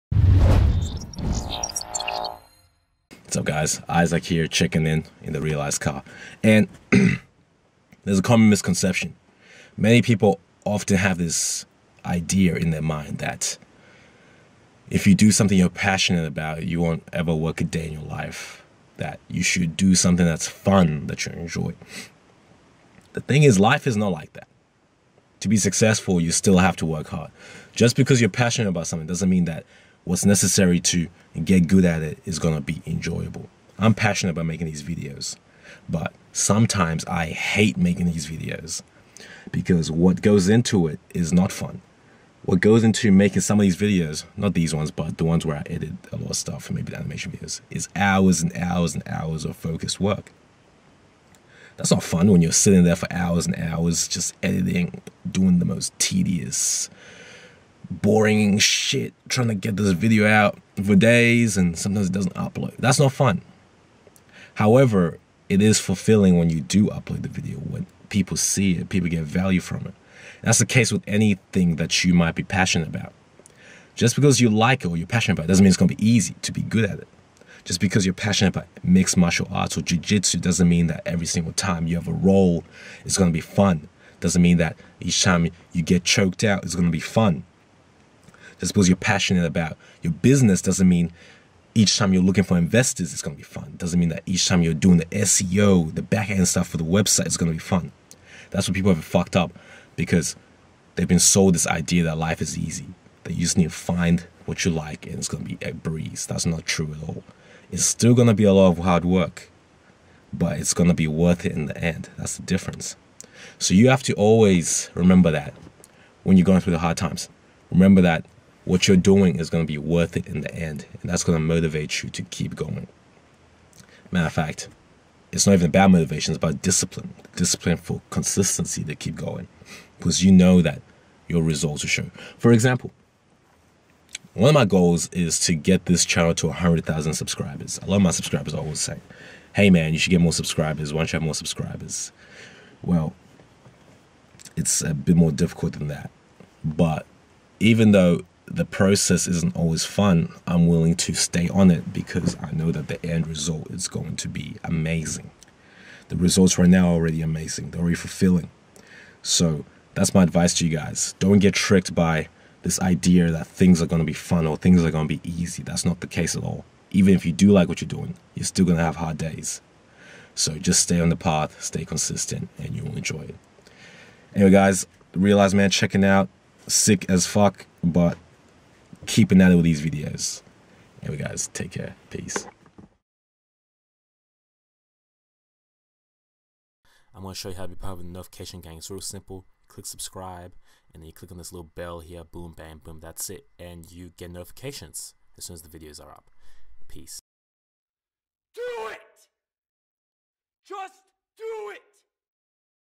Who lied to you? | Truth about your burning passion
If you are passionate about what you do you will never work a day in your life? I call bullshit. regardless about how much passion you have, to reach high levels you are still going to have to put in the work. You are still going to have to go through the drudgery my friend.